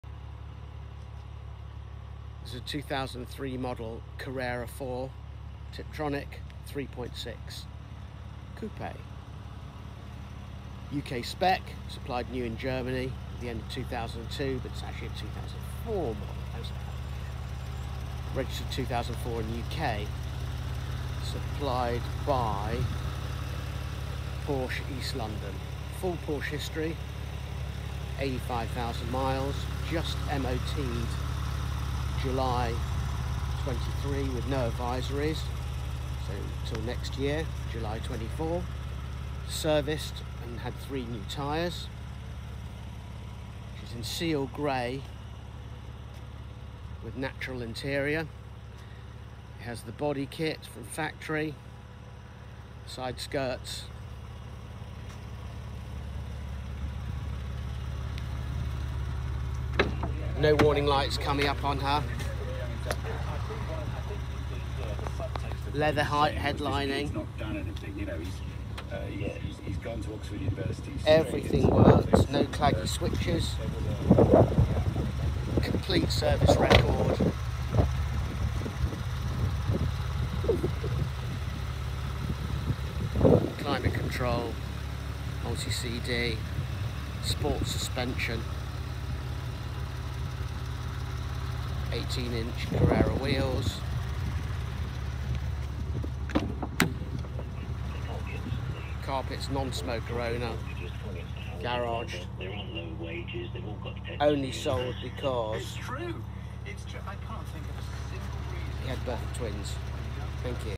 2003 MDL PORSCHE CARRERA 4S 3.6 TIP COUPE ULEZ COMPATIBLE.STUNNING 85,400 MILES FULL HISTORY
A Stunning 2003 MDL UK Spec  Ulez Compliant Carrera 4 S Tiptronic Coupe finished in unmarked Seal Grey Metallic  with Excellent Natural Leather Trim. 85,350 Miles with Full History from new. Just Serviced and MOT Last month. New tyres fitted all invoices. All expired MOT'S.2 Keys. BOSE Upgrade CD/Radio/Navigation. Soft Ruffled Leather. Electric Heated seats/Sunroof. Leather dash and headlining. Porsche Crested seats. 18' Carrera Wheels  with Sports suspension. Factory sideskirts from new. Supplied new to Porsche Stuttgart then retailed by Porsche East London and retailed by the at 4,637 miles on 5/3/04.Last owner 5 years only sold due to arrival of twins. No sticky switches or damaged interior. HPi Clear. Extended Warranty and delivery at extra cost. Condition hard to replicate.